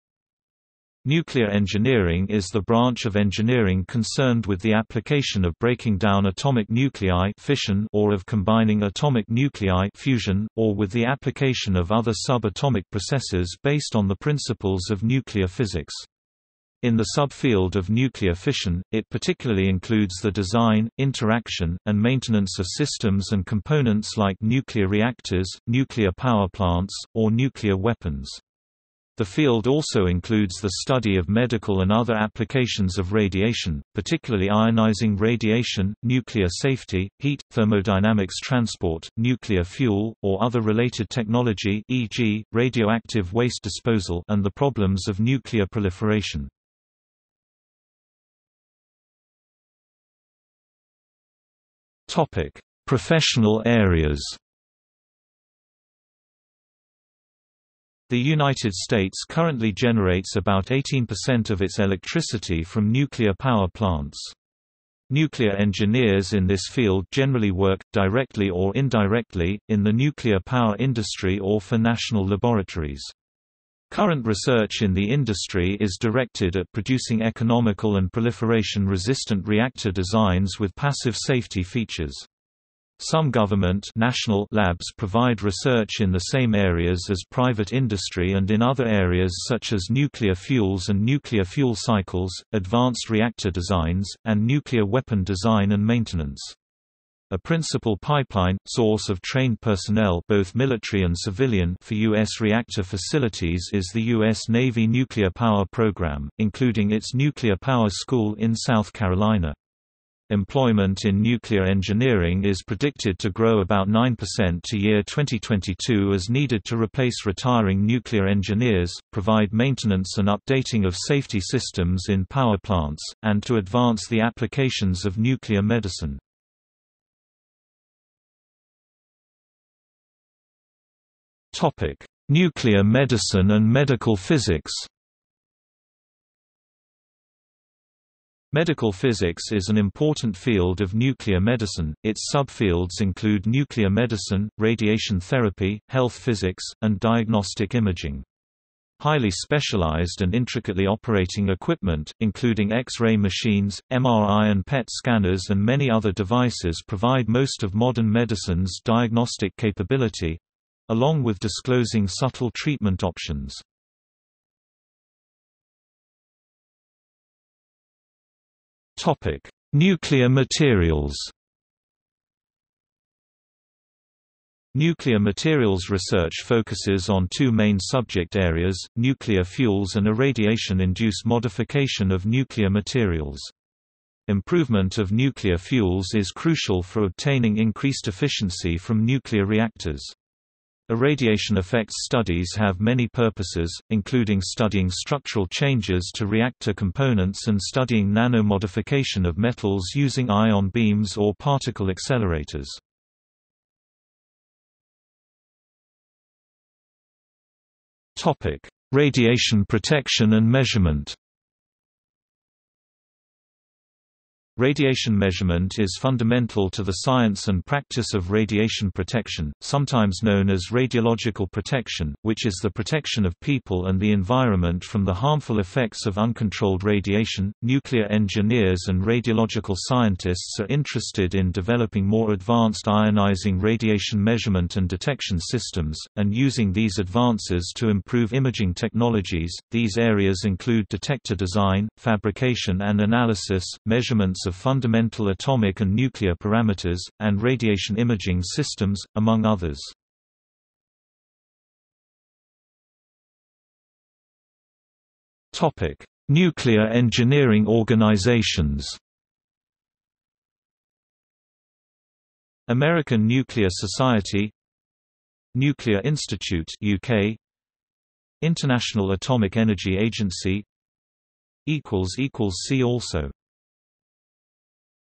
Nuclear engineer | Wikipedia audio article
00:00:48 1 Professional areas
00:02:08 1.1 Nuclear medicine and medical physics
00:02:45 1.2 Nuclear materials
00:03:24 1.3 Radiation protection and measurement
00:04:11 2 Nuclear engineering organizations

- Socrates

SUMMARY
Nuclear engineering is the branch of engineering concerned with the application of breaking down  atomic nuclei (fission) or of combining atomic nuclei (fusion),  or with the application of other sub-atomic processes based on the principles of nuclear physics. In the sub-field of nuclear fission, it particularly includes the design, interaction, and maintenance of systems and components like nuclear reactors, nuclear power plants, or nuclear weapons. The field also includes the study of medical and other applications of radiation, particularly Ionizing radiation, nuclear safety, heat/thermodynamics transport, nuclear fuel, or other related technology (e.g., radioactive waste disposal) and the problems of nuclear proliferation.